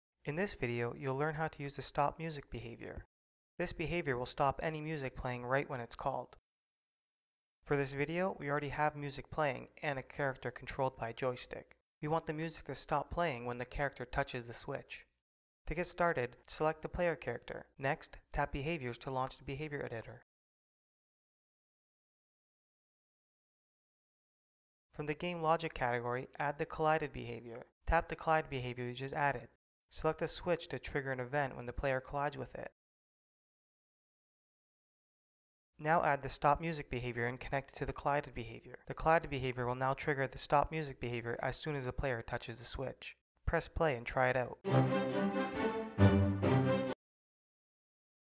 Stop Music Behavior
This video shows you how to stop music playing in your game.